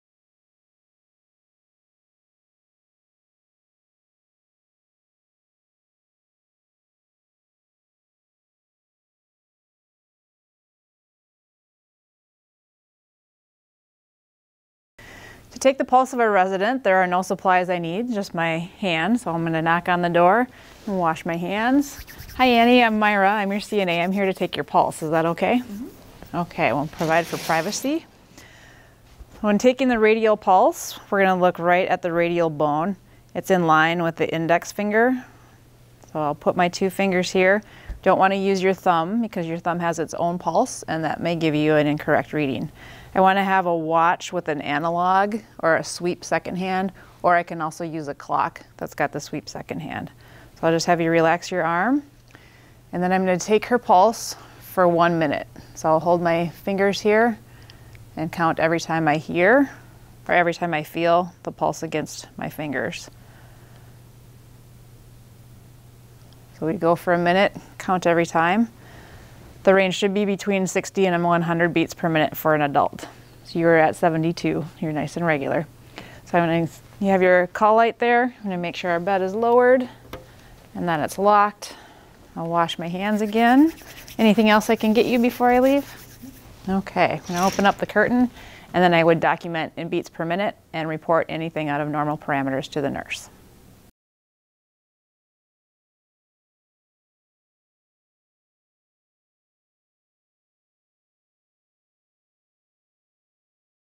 Obtaining a Pulse
This is an instructor demonstration of a nursing skill discussed in the free Nursing Assistant OER textbook. Obtaining a Pulse. [Video]. YouTube.